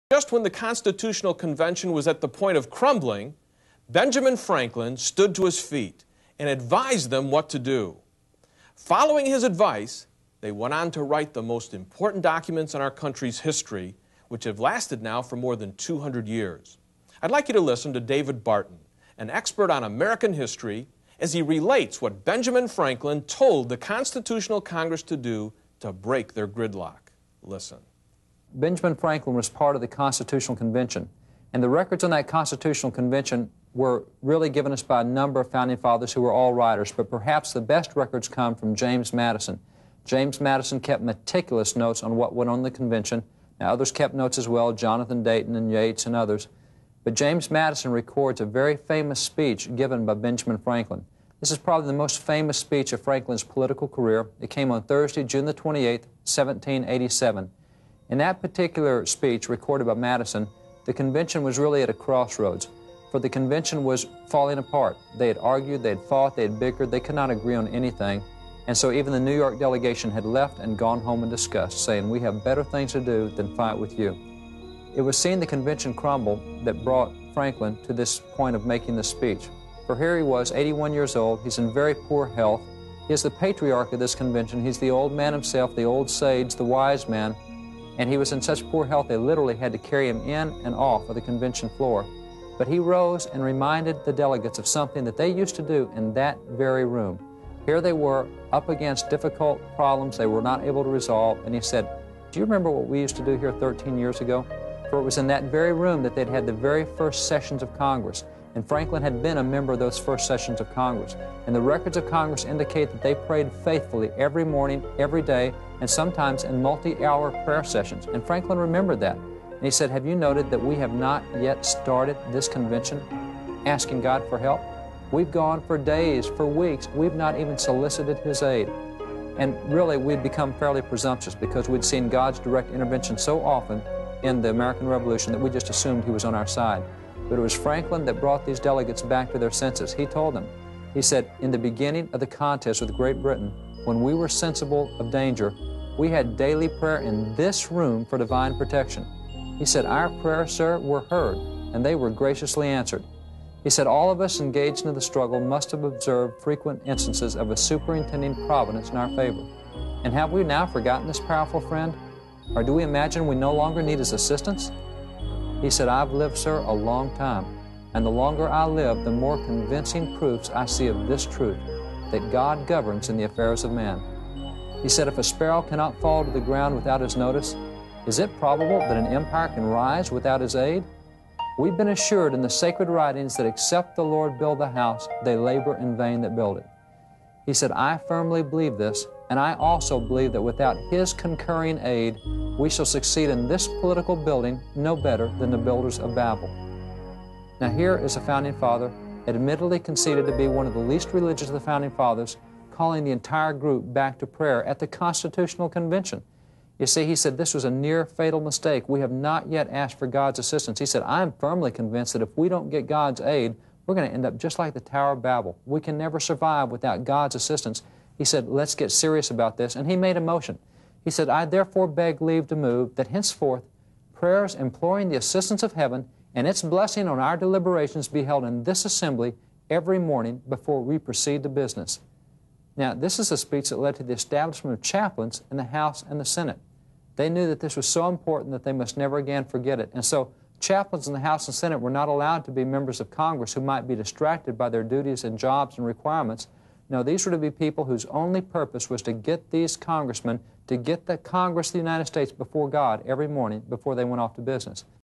Did the Founding Fathers want God, the Bible and prayer to be part of our government?
Did the Founding Fathers want God, the Bible, prayer and Christian morality to be part of our government, schools and our public life?
From our series, "Did our Founding Fathers Intend for America to be Founded as a Christian Nation?"